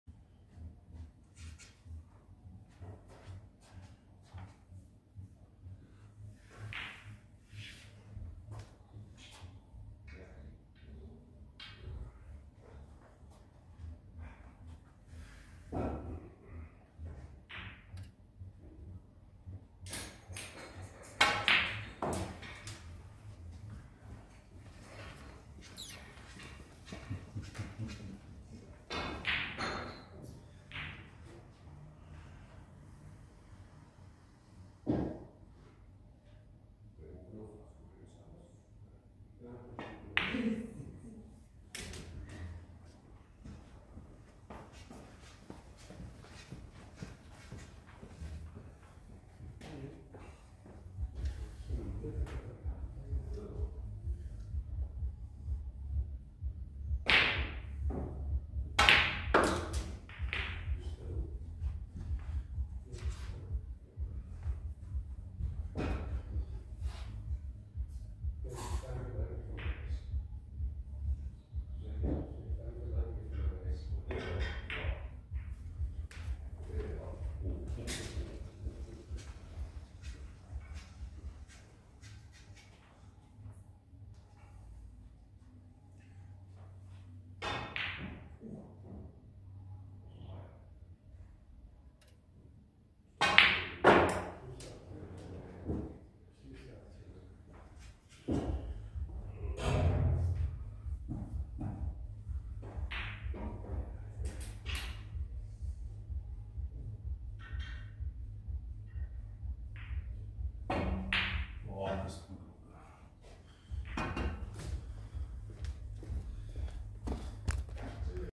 20 punten voor Kristof Smullenberghs - Clear The Colours - Summer Challenge 2024
Ook Kristof blijft op 20 punten hangen net zoals al de rest die deze geprobeerd hebben.

Maar hoe dan ook, een voorlopige 3de plaats voor Kristof in de Niveau 3 ranking.